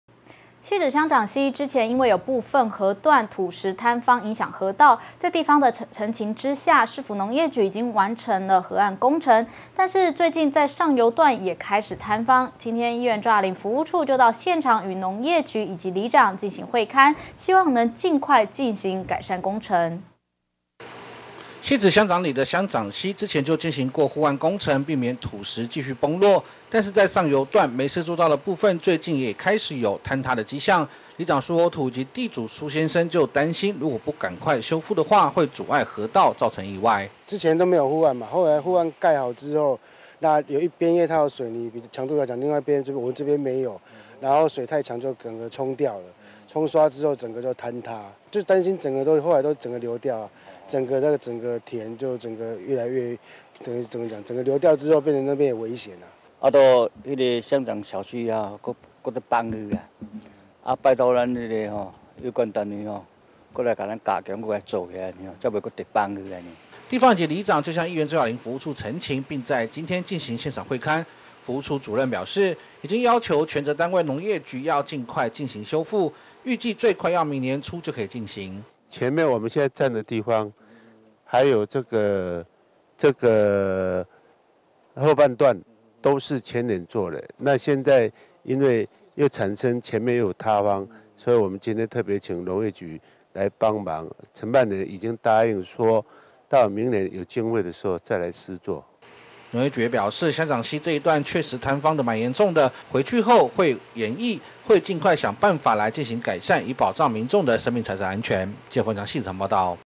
1061123觀天下新聞HD03 汐止區鄉長溪邊坡滑落塌陷 地方盼盡快修復
汐止鄉長里的鄉長溪之前因為有部分地段土石坍方影響河道，在地方的陳情下，市府農業局已經完成河岸工程，但上游段最近也開始坍方，在今(23)日議員周雅玲服務處就到現場與農業局以及里長進行會勘，希望能盡快進行改善工程。